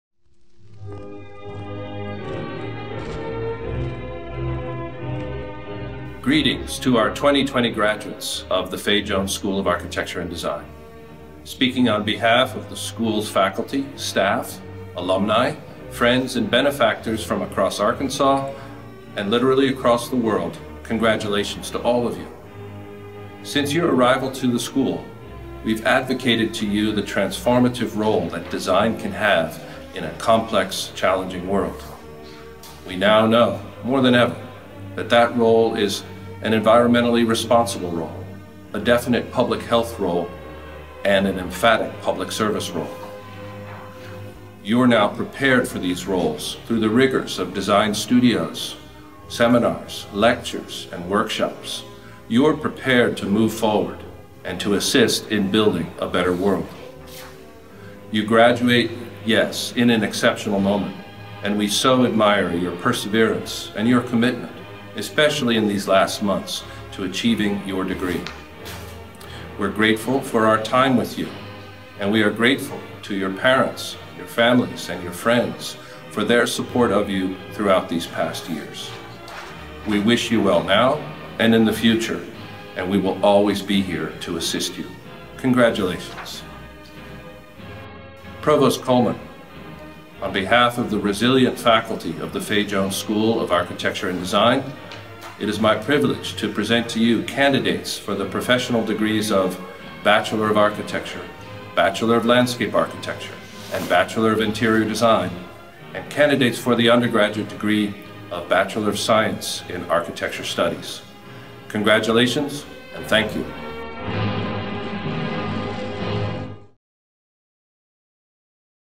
Dean Peter MacKeith, Fay Jones School of Architecture and Design
Commencement message to the spring 2020 graduates of the University of Arkansas.